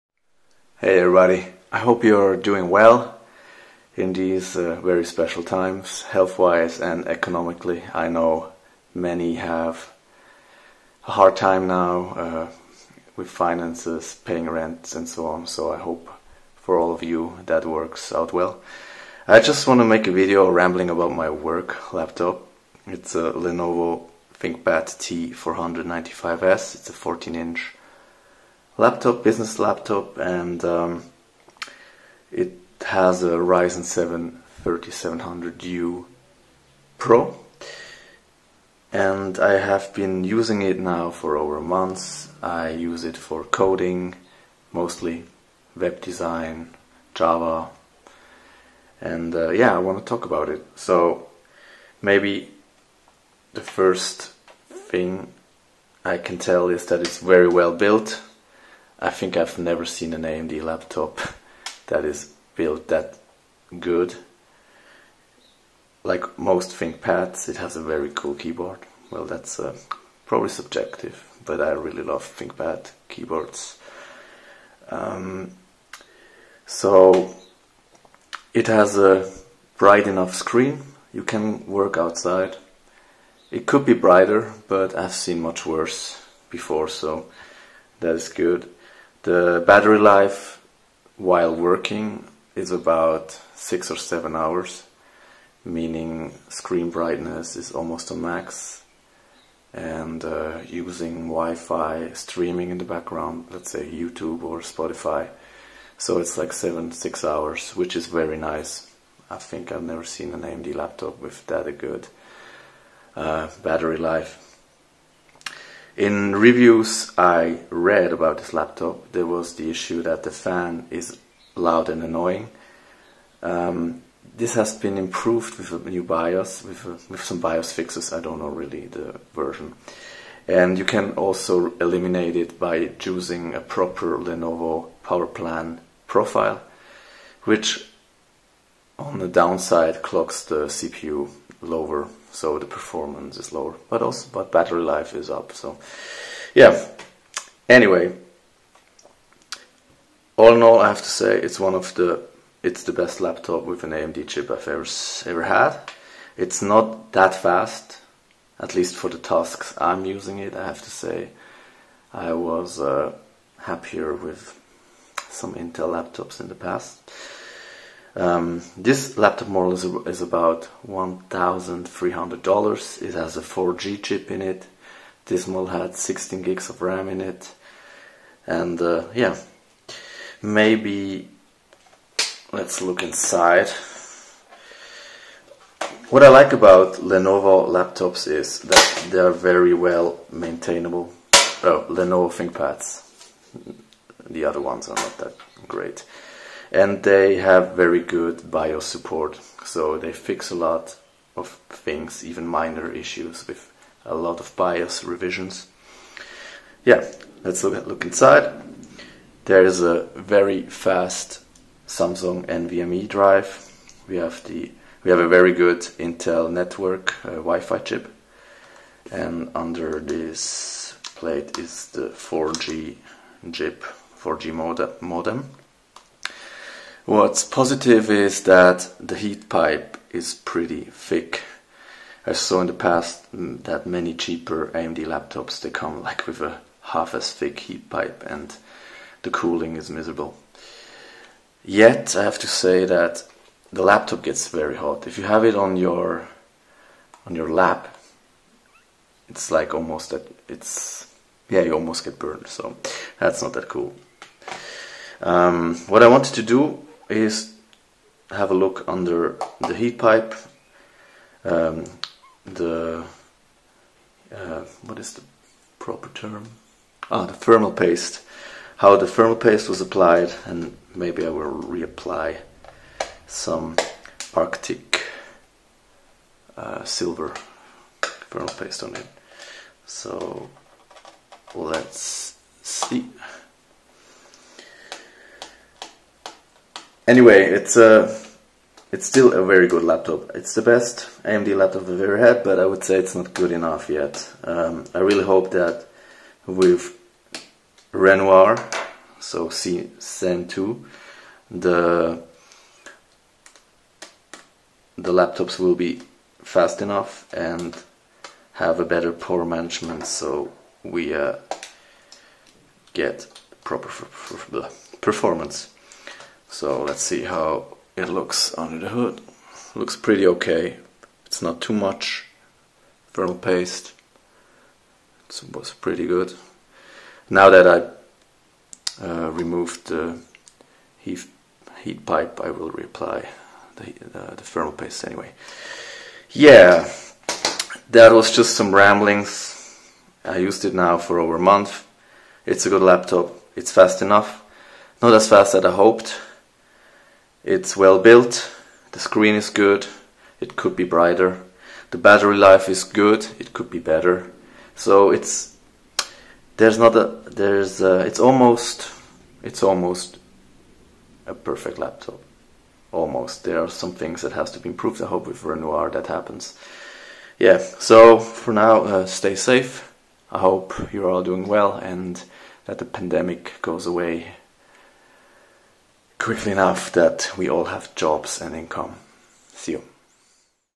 Lenovo Thinkpad T495s - One of the Best, But not good enough.
Lenovo Thinkpad T495s Review
Probably Ryzen 4000 will do the trick.